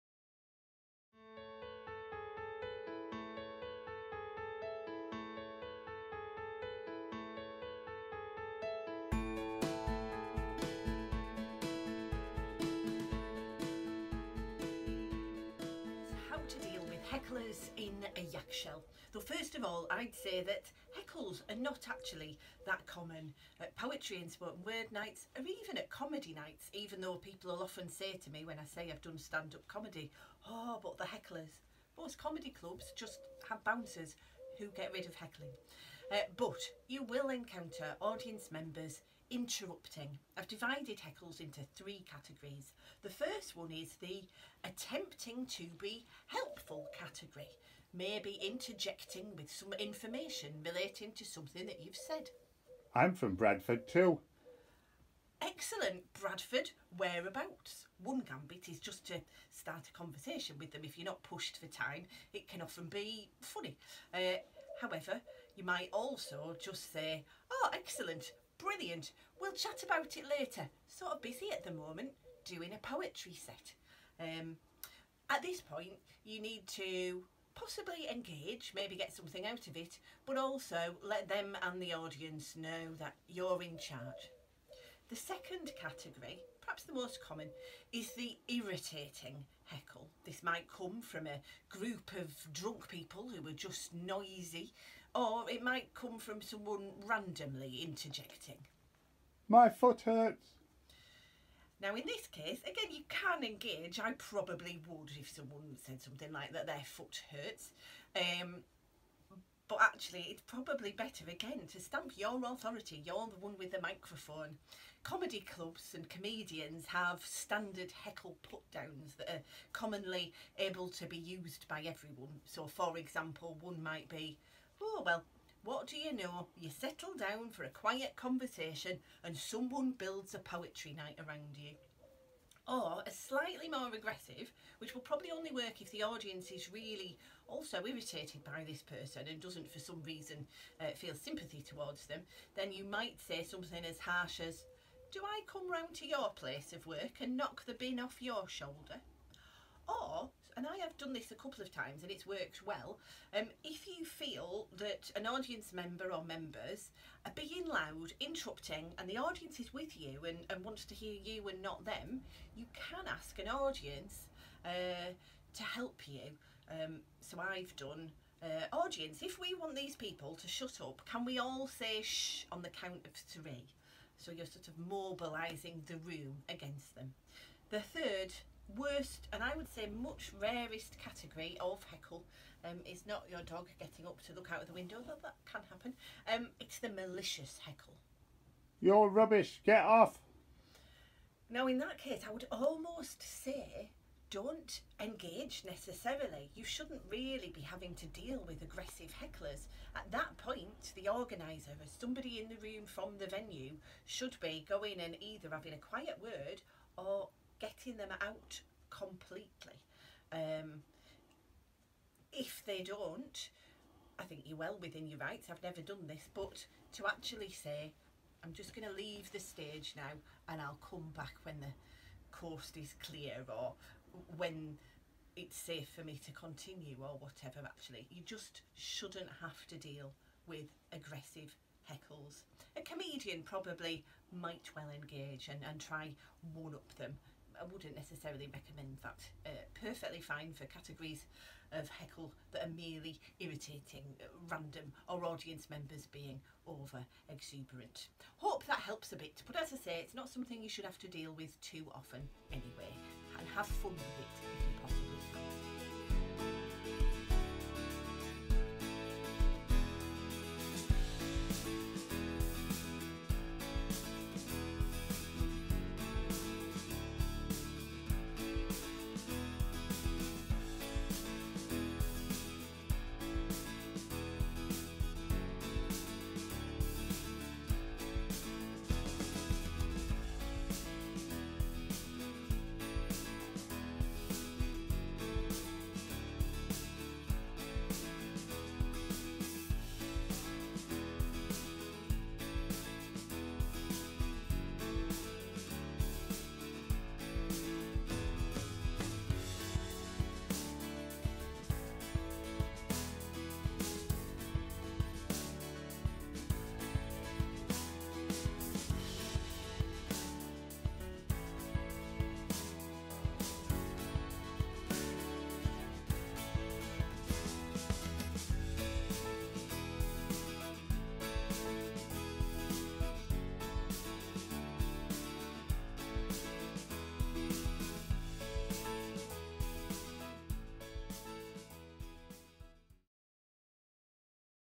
Hip Yak Poetry School - In A Yak Shell - Kate Fox on Hecklers
The Hip Yak Poetry School presents a series of online teaching tutorials for spoken word poets. In this film Kate Fox explains how to deal with hecklers.

To find out about the Hip Yak Poetry School and to watch all our online films please